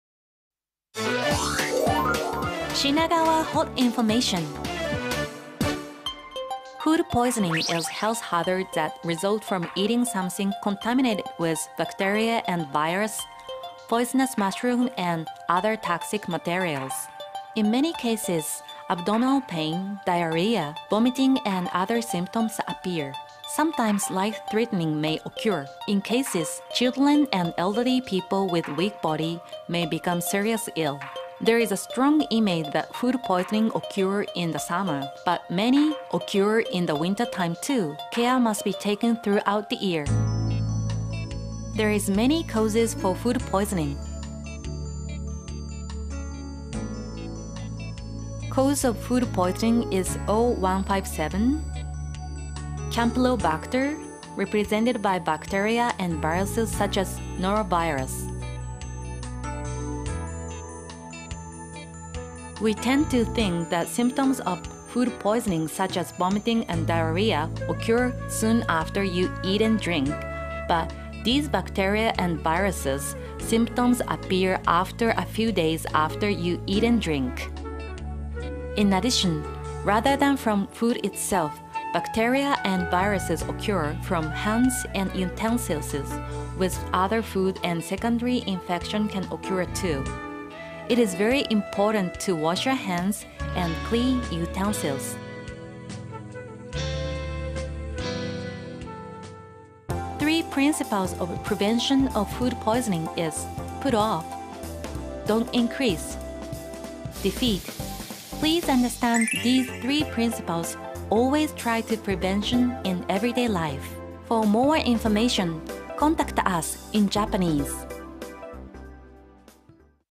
【英語(English)】食中毒予防 食中毒って？
食中毒とは、細菌やウイルスに汚染されたものや、毒キノコなどの有毒物を食べた結果起こる健康被害のことです。
Food poisoning is, contaminated with bacteria and viruses, and health hazards that result from eating poisonous mushrooms and other toxic materials.

多くの場合、腹痛、下痢、嘔吐などの症状があらわれ、ときには、生命に関わることもあります。特に、身体の弱い子供やお年寄りは、重症となってしまう場合があります。
In many cases, abdominal pain, diarrhea, vomiting and other symptoms appear,
sometimes life-threatening may occur. In cases, children and elderly people
with weak body, may become serious ill.

食中毒は夏場に起こるイメージが強いですが、冬場でも多く発生するため、
年間を通して注意が必要です。
Food poisoning has a strong image that occur in the summer, but many occur in the winter time too, care must be taken throughout the year.

食中毒の原因となるものは、いろいろあります。
There is many cause for food poisoning.

食中毒の原因はO－157、カンピロバクターに代表される細菌の仲間、
そしてノロウイルスなどのウイルスの仲間です。
Cause of food poisoning is O-157, represented by bacteria Campylobacter,
and viruses such as Norovirus.

食中毒というと飲食して、すぐに下痢・嘔吐といった症状が出ると思われがちですが、これらの細菌やウイルスの場合、飲食して数日後に症状が出る事が多いです。
Food poisoning  symptoms is often thought to occur soon after you eat and drink, such as vomiting and diarrhea symptoms, but these bacteria and viruses, symptoms appear after a few days after you eat and drink.

また、細菌やウイルスは食品自体からではなく、手や調理器具から、他の食品に付き、二次的に感染することも多いため、手洗い・調理器具の洗浄・消毒が大変重要です。
In addition, bacteria and viruses does not occur from food itself, rather than from hands and utensils with other foods,  secondary infection can occur too, it is very important to wash your hand and clean utensils.

食中毒予防の３原則は「つけない」「増やさない」「やっつける」。
この３原則をしっかり理解し、毎日の生活の中で予防を心がけましょう。
3 principles of prevention of food poisoning is "put off", "not increase," "defeat".
Please understand these 3 principles, always try prevention in everyday life.